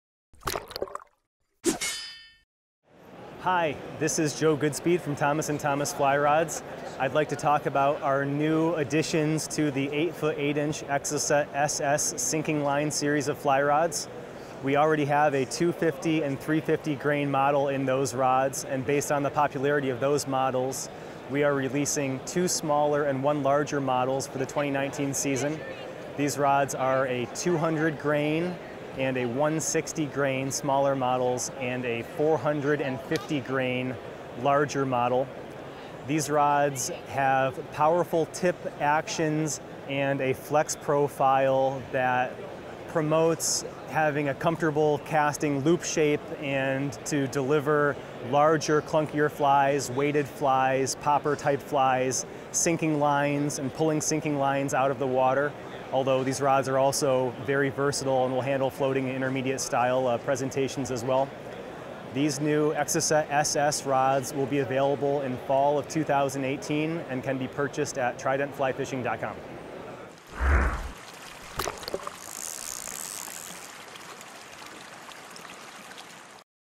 Thomas & Thomas Exocett SS Fly Rod | Joe Goodspeed Insider Review
Developed to meet the challenges of the most demanding species and destinations, T&T’s Exocett Series of high-performance saltwater fly rods are infused with the exceptional strength and dampening characteristics of our proprietary StratoTherm Resin.

Support us producing more of these videos by making your next gear purchase through us

Got Questions?